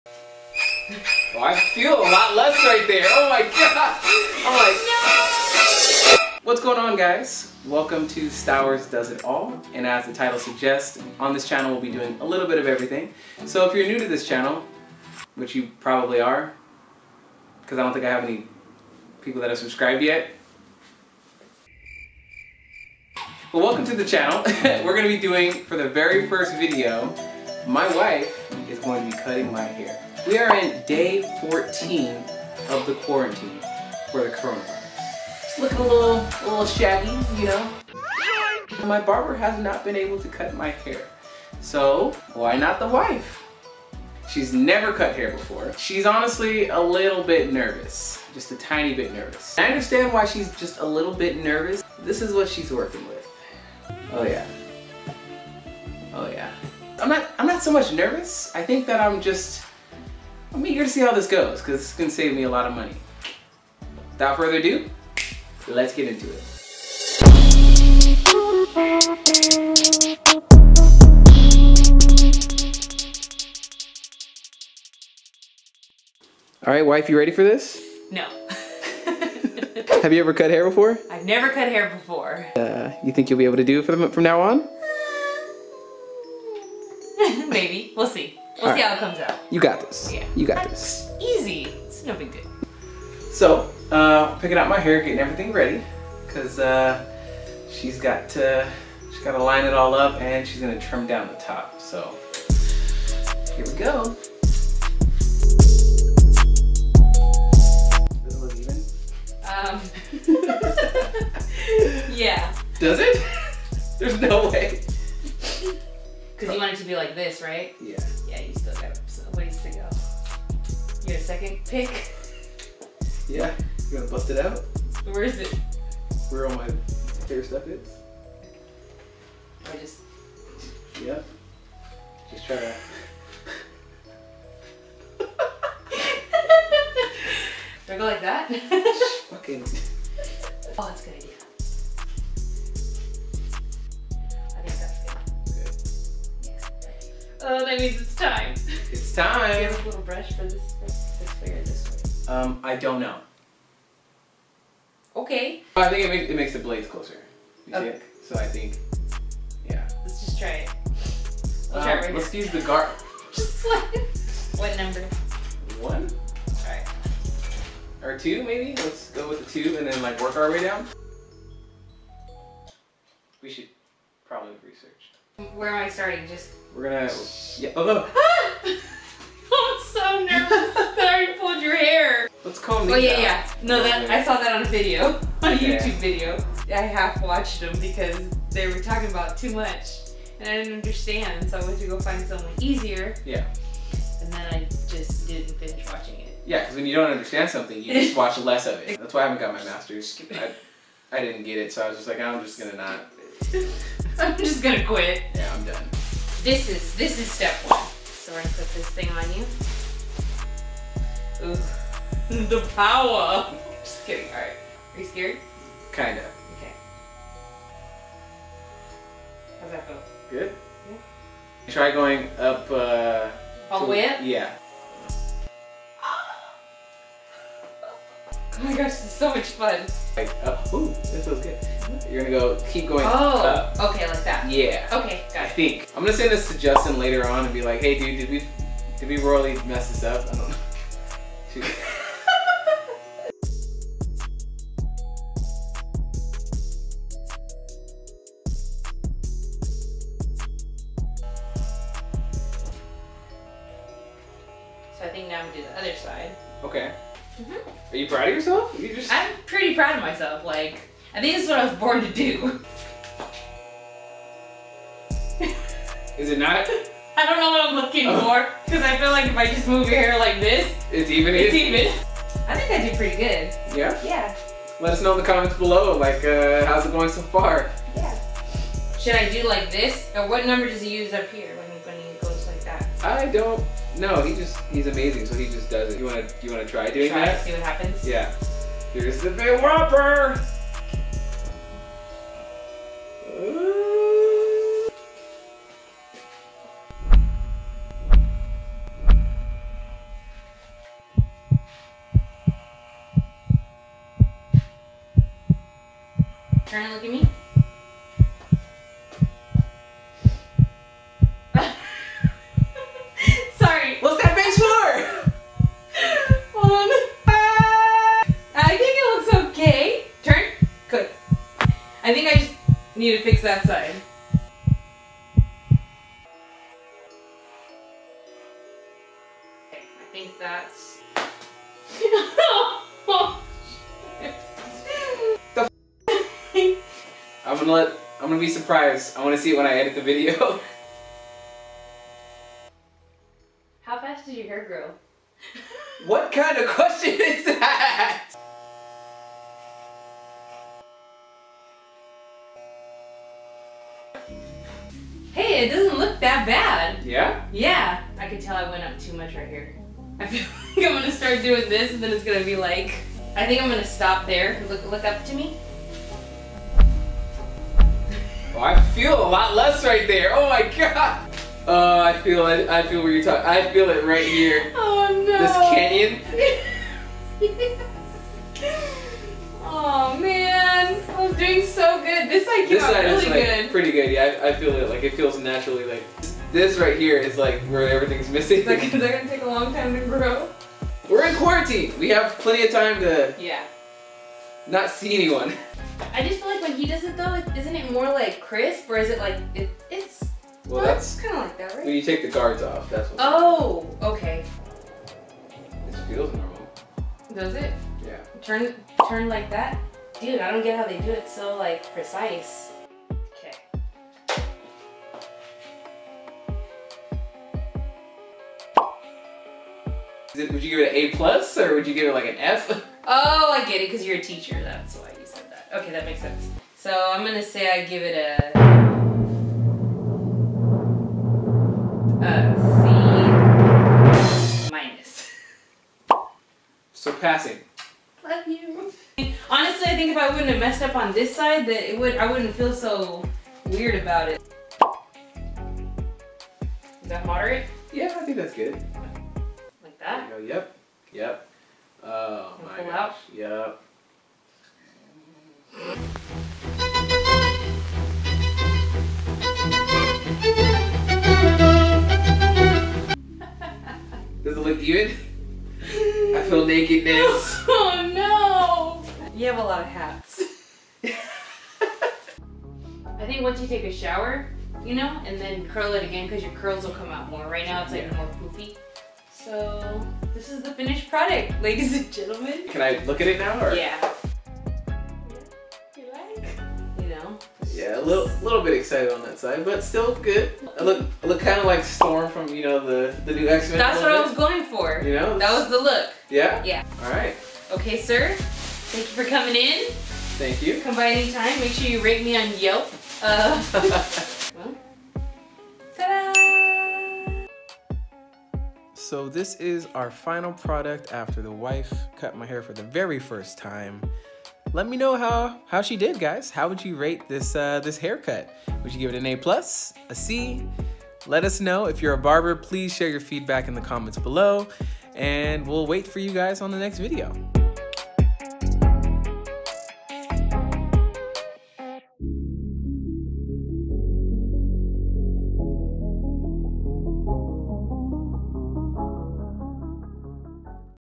Wife Cuts My Hair Since We're Quarantined in the House! 🦠🏡
For the first video of my channel, my wife CUTS MY HAIR!!
My normal barber can't get to me... sooooooooo HOW'D SHE DO??

California ordered a shelter-in-place on March 16th. aaanndddd we're going a little stir crazy.